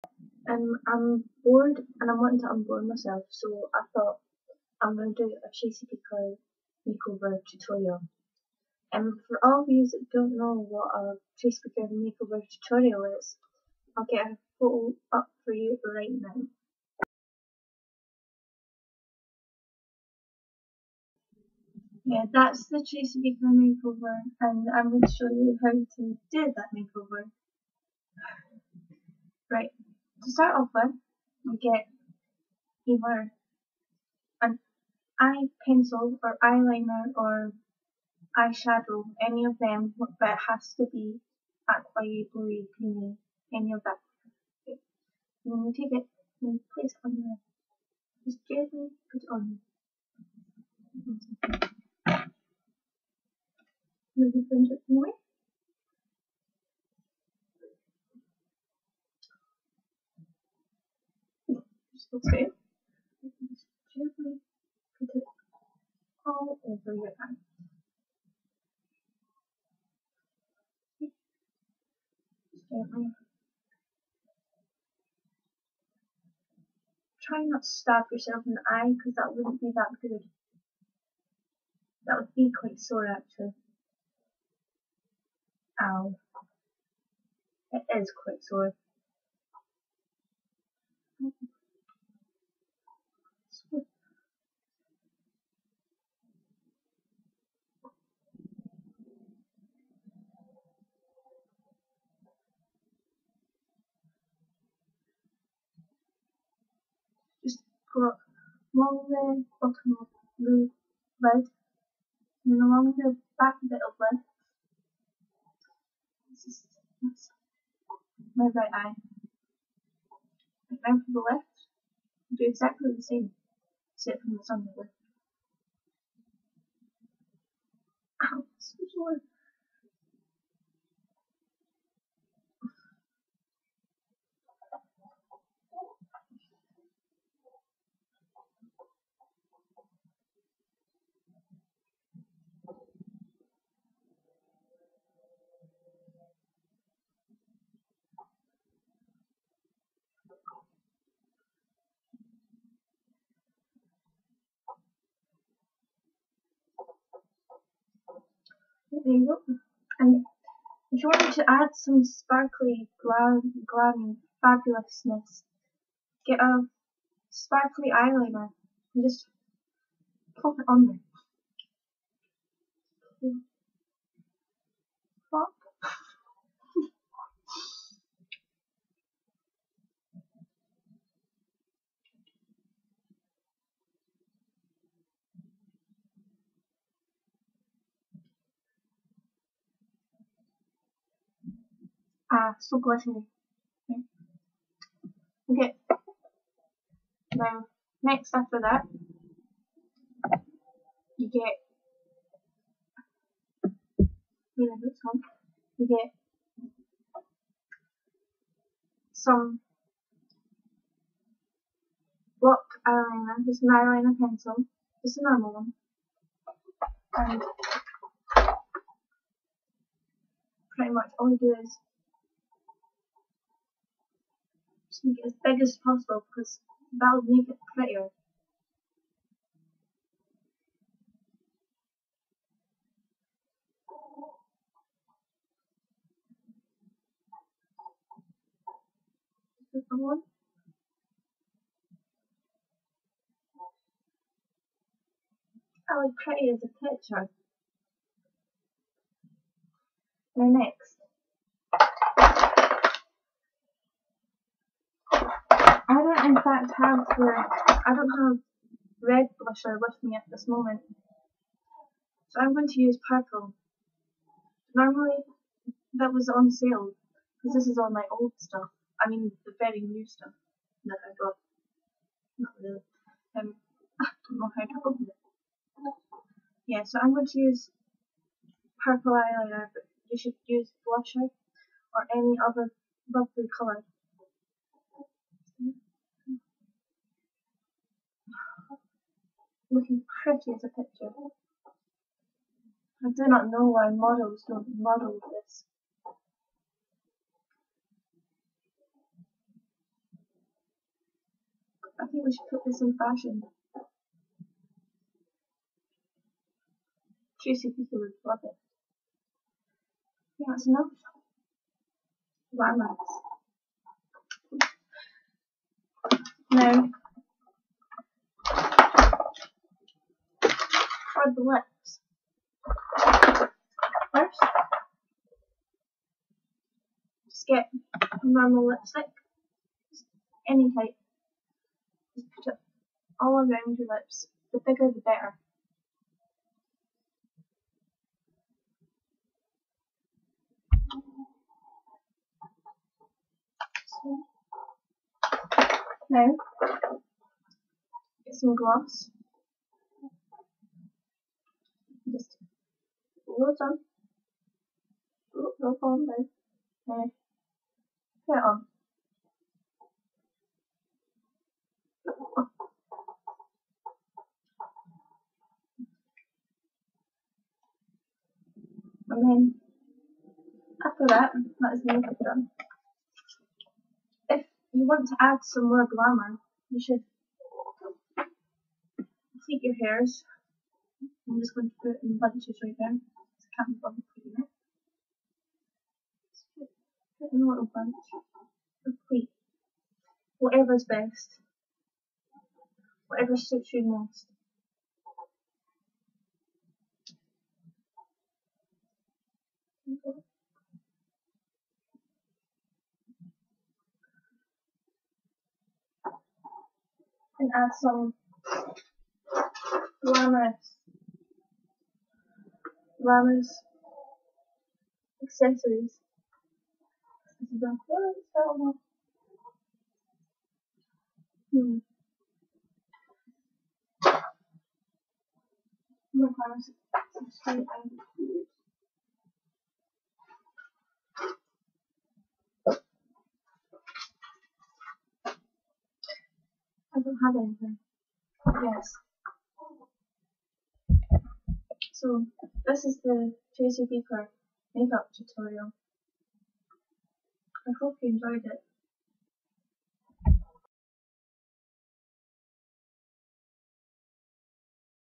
Tracy Beaker's "makeover" Tutorial (For fun)
Did this out of boredom lololol don't judge me . All of the makeup was stuff from magazines from when I was little/was never gonna use them any more.